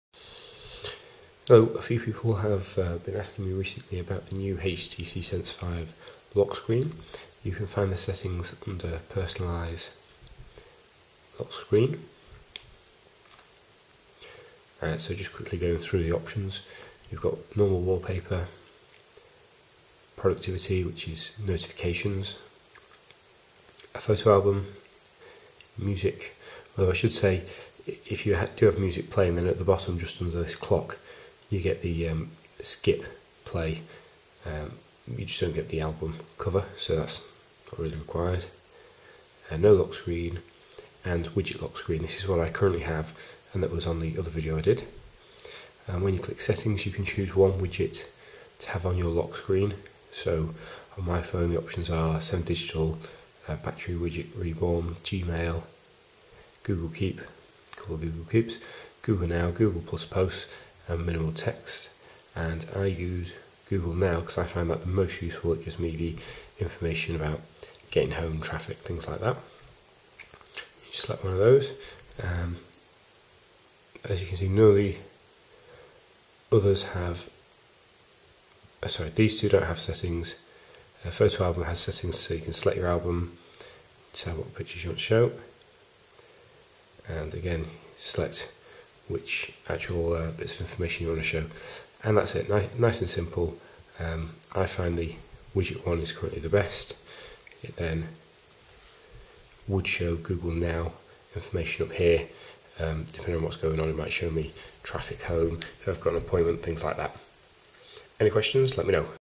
HTC Sense 5 Lock Screen Overview
A quick overview of the new lock screen options on HTC Sense 5 interface.